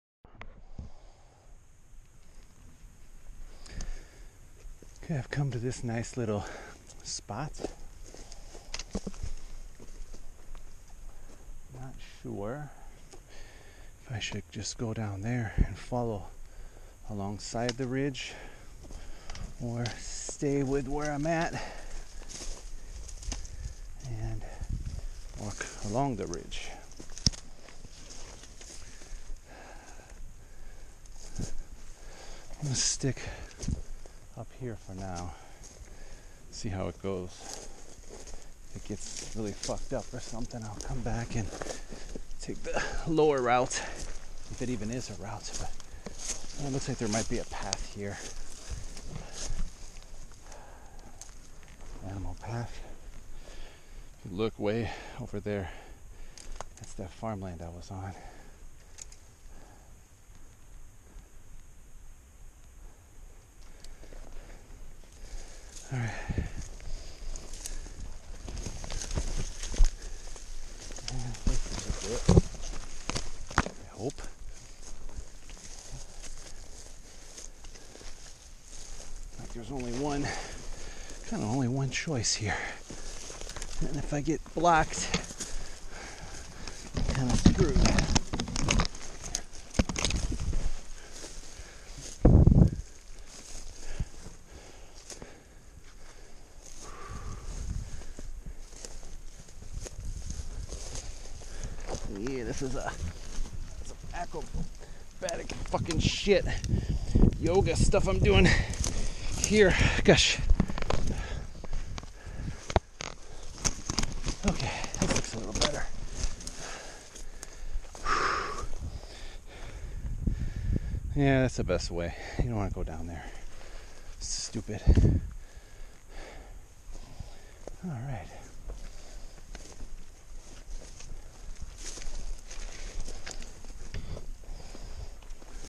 Ridge Section of Hike from Neiwenshan to Bacengbamoshan - 巴層巴墨山途經內文山
A short section of the ridge hike between the two peaks.